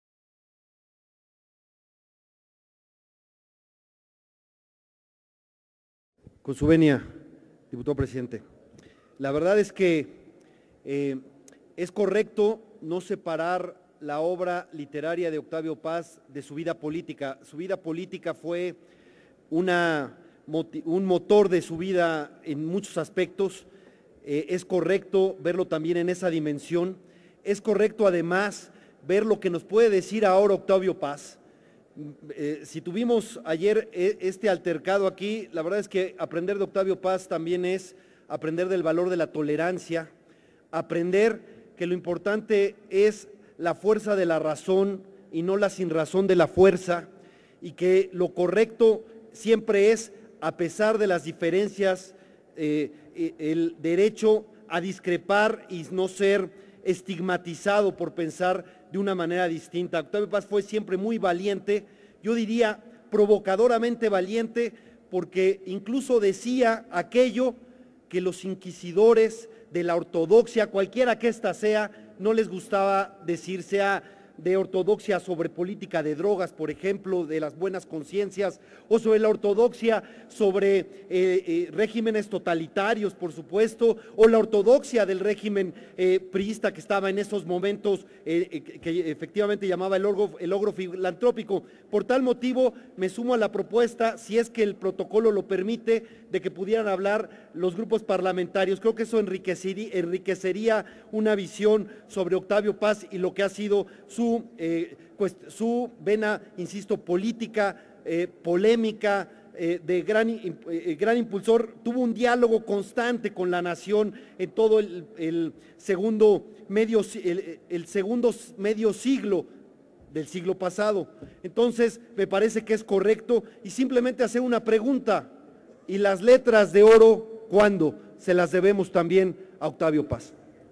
Dip. @ferbelaunzaran sobre acuerdo de la JUCOPO relativo a Sesión Solemne http://
INTERVENCIÓN EN TRIBUNA DEL DIPUTADO FEDERAL FERNANDO BELAUNZARÁN MÉNDEZ, PARA FIJAR EL POSICIONAMIENTO DEL GPPRD RESPECTO DEL ACUERDO DE LA JUNTA DE COORDINACIÓN POLÍTICA POR EL QUE SE ESTABLECE EL FORMATO PARA LA SESIÓN SOLEMNE QUE CONMEMORA EL CENTENARIO DEL NATALICIO DE OCTAVIO PAZ

Con su venia, diputado presidente. La verdad es que es correcto no separar la obra literaria de Octavio Paz de su vida política. Su vida política fue un motor de su vida en muchos aspectos. Es correcto verlo también en esa dimensión. Es correcto además ver lo que nos puede decir ahora Octavio Paz.

Si tuvimos ayer este altercado aquí, la verdad es que aprender de Octavio Paz, también es aprender del valor de la tolerancia, aprender que lo importante es la fuerza de la razón y no la sinrazón de la fuerza y que lo correcto siempre es —a pesar de las diferencias— el derecho a discrepar y no ser estigmatizado por pensar de una manera distinta.

Octavio Paz fue siempre muy valiente. Diría provocadoramente valiente, porque incluso decía aquello que los inquisidores de la ortodoxia, cualquiera que esta sea, no les gustaba decir, sea de ortodoxia sobre política de drogas, por ejemplo, de las buenas consciencias, o de la ortodoxia sobre regímenes totalitarios, por supuesto, o la ortodoxia del régimen priista que estaba en esos momentos y que efectivamente llamaba el 'Ogro Filantrópico'.

Por tal motivo, me sumo a la propuesta, si es que el protocolo lo permite, de que pudieran hablar los grupos parlamentarios. Creo que eso enriquecería una visión sobre Octavio Paz y lo que ha sido su pena, insisto, política, polémica, de gran impulsor. Tuvo un diálogo constante con la nación en todo el segundo medio siglo, del siglo pasado. Entonces, me parece que es correcto.

Y simplemente hacer una pregunta, y las letras de oro, ¿cuándo? Se las debemos también a Octavio Paz.